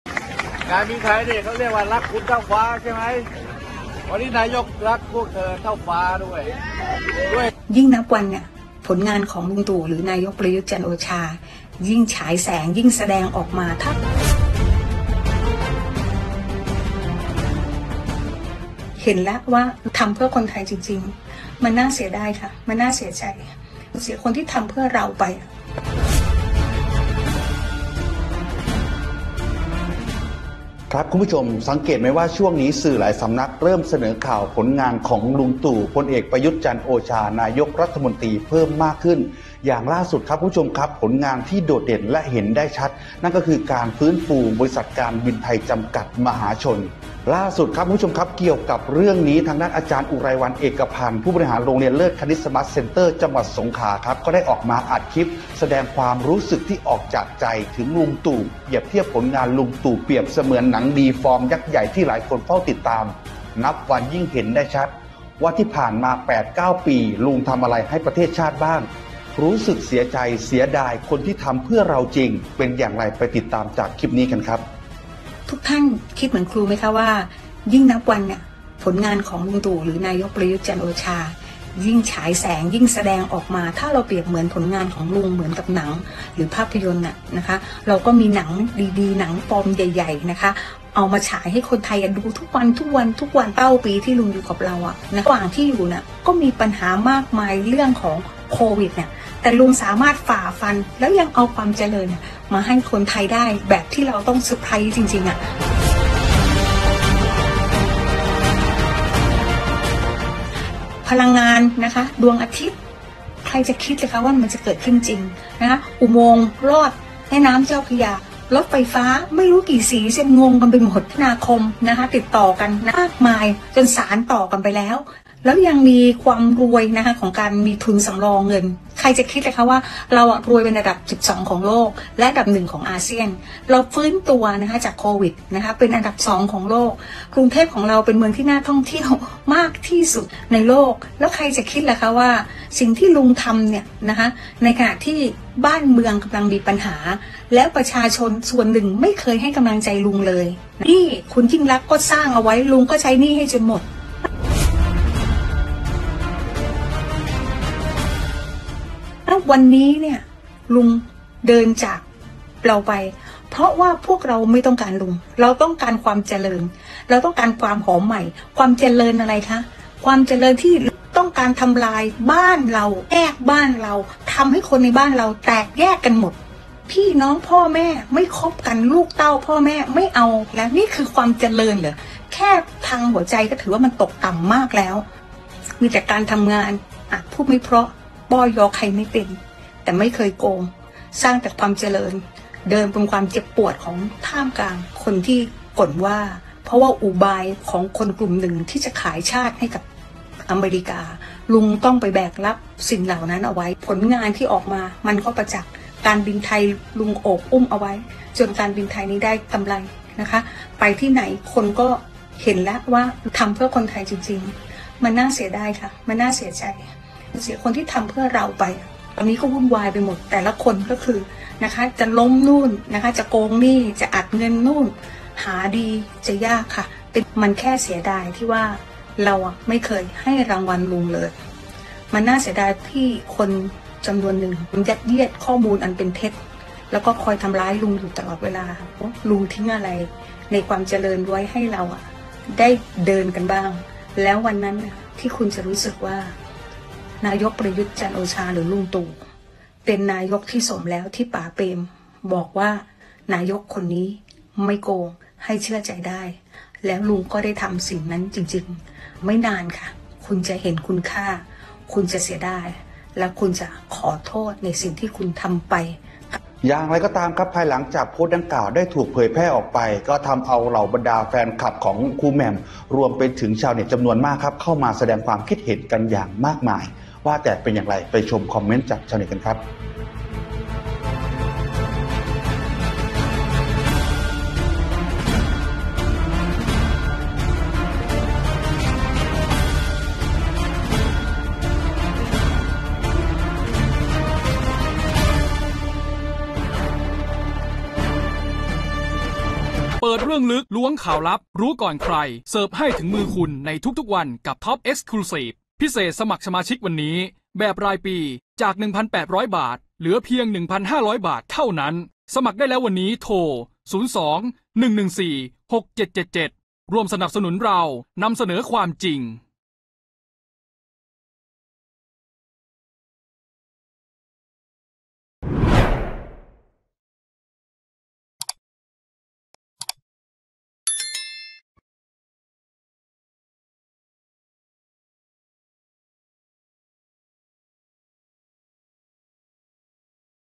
ออกจากใจ "อ.อุไรวรรณ" เสียดาย "ลุงตู่" ผลงาน 8 ปี นับวันยิ่งเห็นได้ชัด | HotSocial
ออกจากใจ “อ.อุไรวรรณ” เปรียบผลงาน “ลุงตู่” เสมือนหนังฟอร์มยักษ์ นับวันยิ่งเห็นได้ชัด 8 ปี ลุงทำอะไรให้ประเทศชาติบ้าง รู้สึกเสียใจ เสียดาย คนที่ทำเพื่อเราจริง